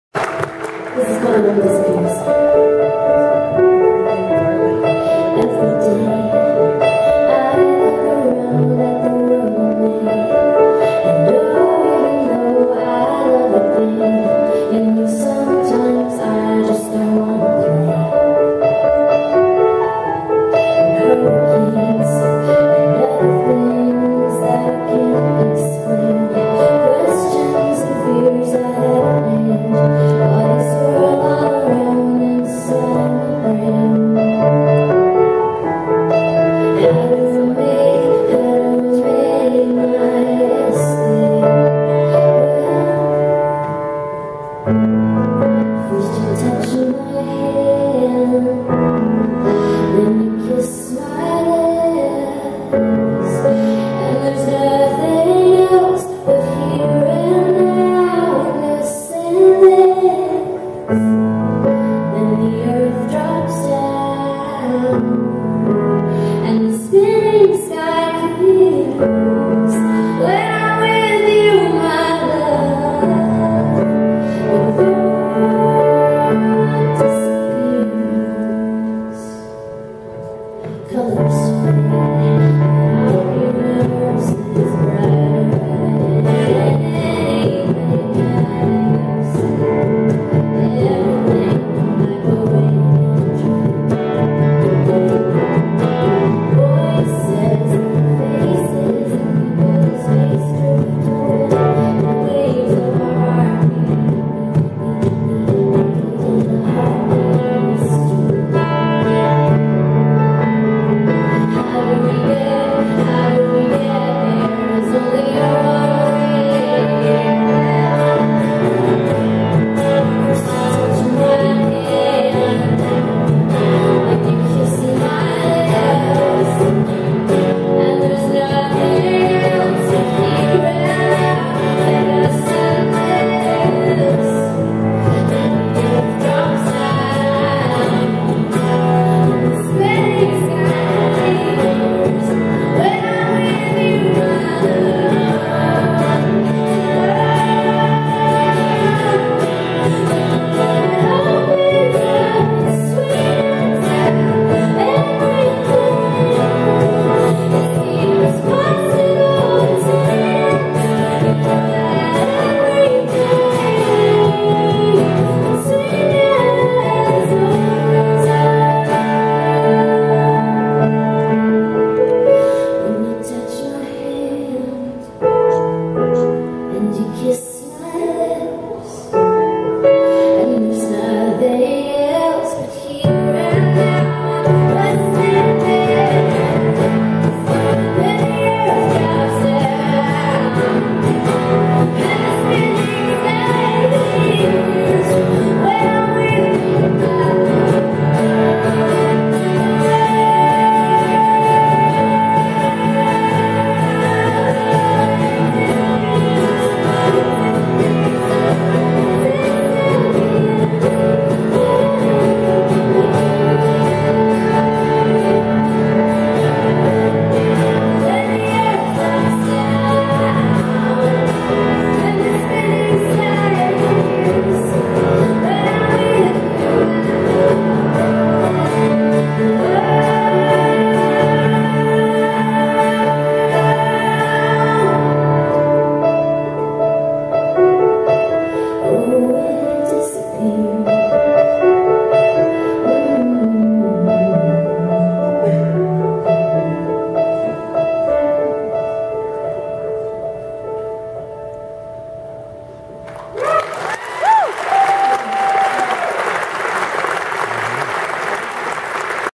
Cassandra Kubinski Artists for Autism Buffalo 4-16-16
Love this girl!  She is amazing on the piano and has a beautiful voice!